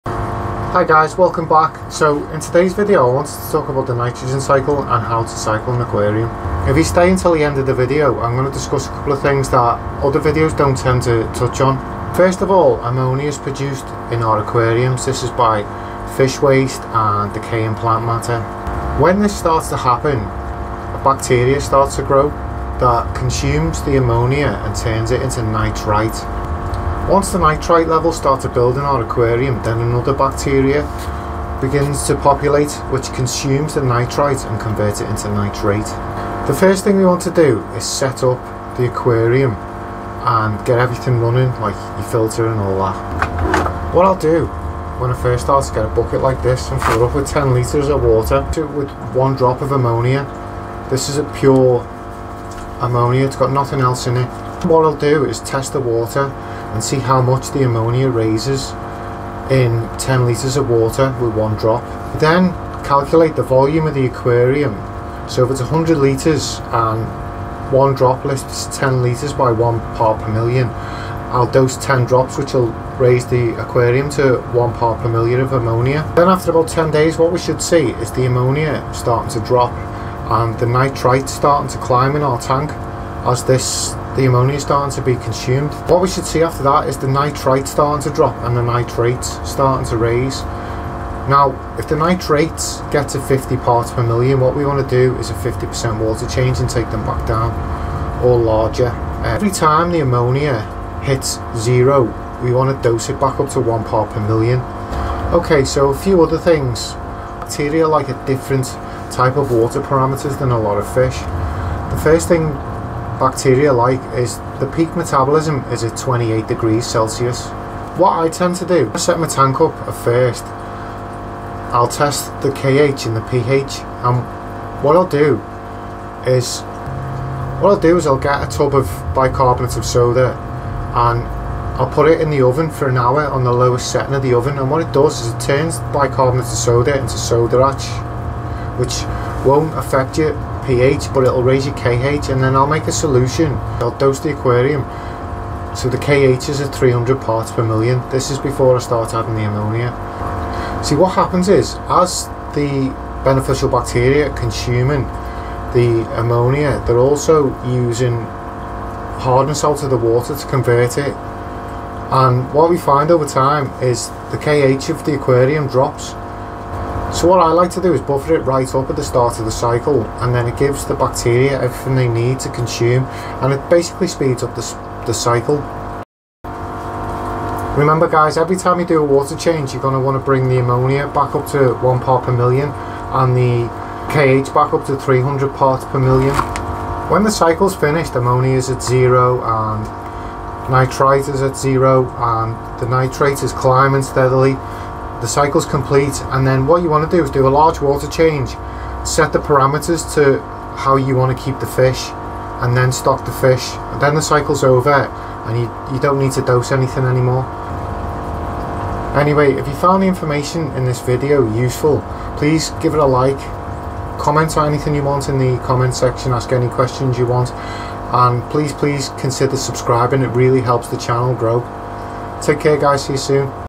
How I Cycle an Aquarium | The Nitrogen Cycle Explained.
In today's video, I talk through the nitrogen cycle and explain in a little depth how I cycle my aquariums.